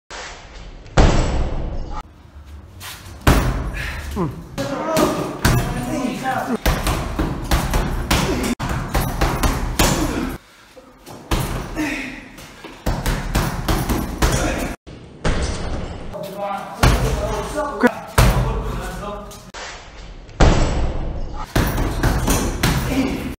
Kicking boxing Training my Jeetkunedo Piero Piromallo inspired by Bruce Lee martial arts
kicking boxing